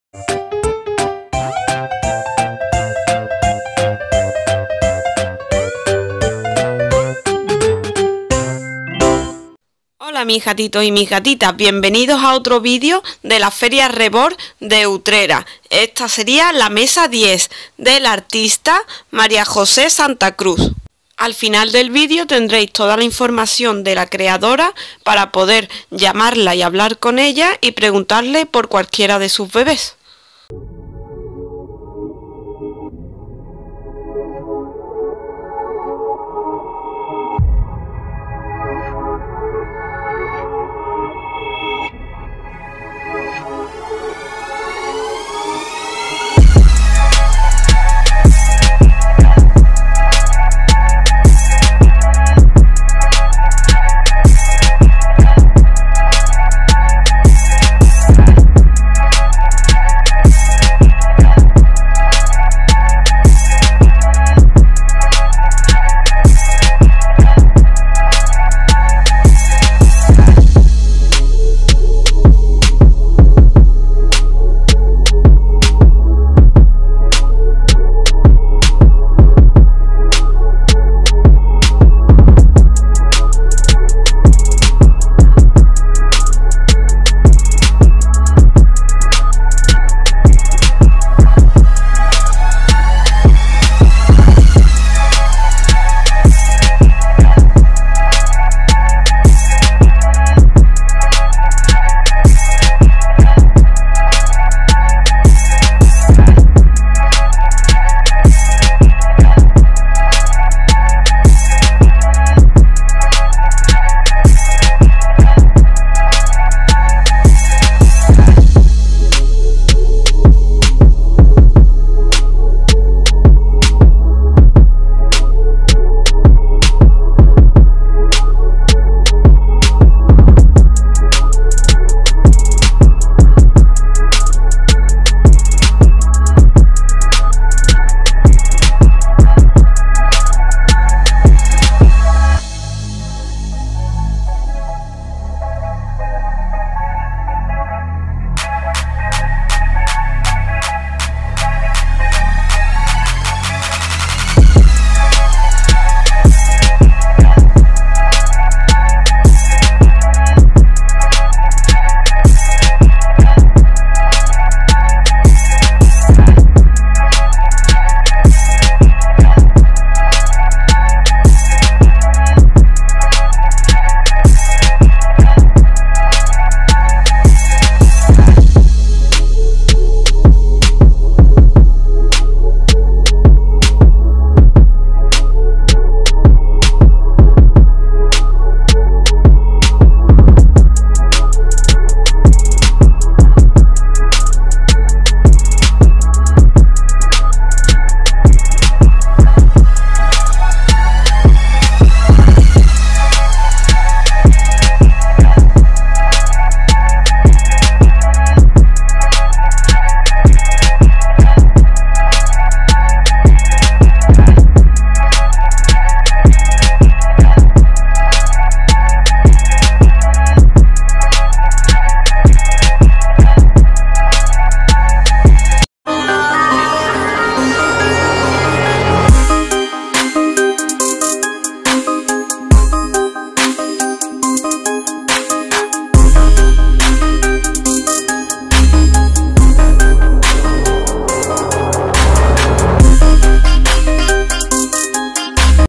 Feria Reborn Utrera: Reborns Bebes Estrellitas y Luceritos
Muñecos reborn pintados a manos de vinilo.
( toda la información del vendedor al final del vídeo) MESA 10.